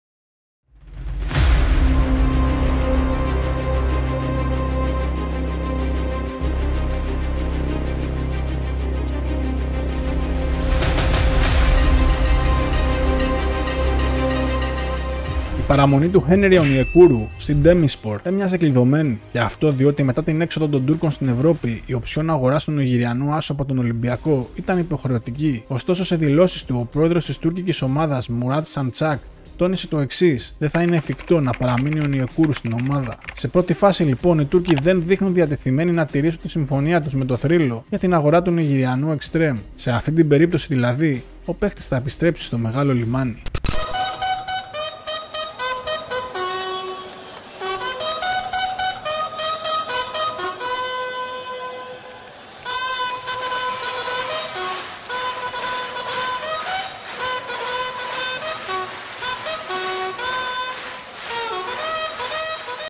Ολυμπιακός Μεταγραφές & Χένρι Ονιεκούρου | Olympiacos Fc Θρύλος Είσαι ΟΣΦΠ Νέα Ποδόσφαιρο
Ολυμπιακός Μεταγραφές & Χένρι Ονιεκούρου (Olympiacos Fc Θρύλος Είσαι ΟΣΦΠ Νέα Ολυμπιακού Τώρα στο Ποδόσφαιρο & Ρεπορτάζ Ολυμπιακού): Η παραμονή του Χένρι Ονιεκούρου στην Ντεμίρσπορ έμοιαζε «κλειδωμένη». Και αυτό, διότι μετά την έξοδο των Τούρκων στην Ευρώπη, η οψιόν αγοράς του Νιγηριανού άσου από τον Ολυμπιακό ήταν υποχρεωτική.

Ωστόσο, σε δηλώσεις του, ο πρόεδρος της τουρκικής ομάδας, Μουράτ Σαντσάκ, τόνισε: «Δεν θα είναι εφικτό να παραμείνει ο Ονιεκούρου στην ομάδα».

Σε πρώτη φάση, λοιπόν, οι Τούρκοι δεν δείχνουν διατεθειμένοι να τηρήσουν τη συμφωνία τους με τον Θρύλο, για την αγορά του Νιγηριανού εξτρέμ. Σε αυτή την περίπτωση, δηλαδή, ο παίκτης θα επιστρέψει στο μεγάλο λιμάνι.